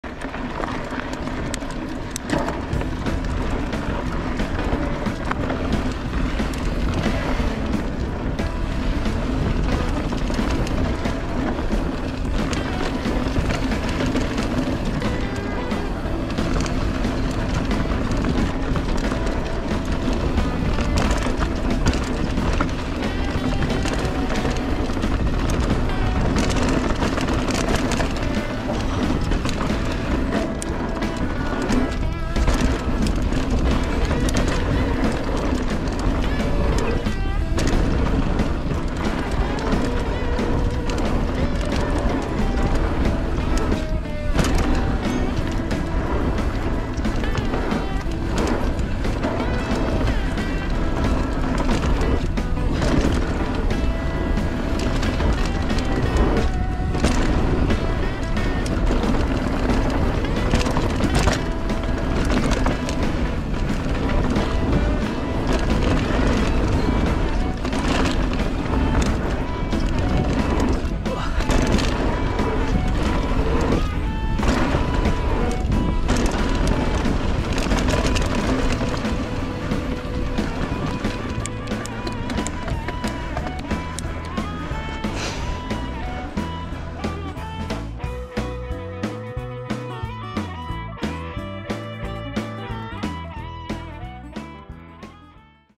[Daisy Hill] Bronze Wing | Sep 2023
Another boring POV video, down flowy 'Bronze Wing' at Daisy Hill.
Uploading mainly for documentation purposes -- something to look again in the future to see how I progress.
Another purpose, to keep working on video editing skill.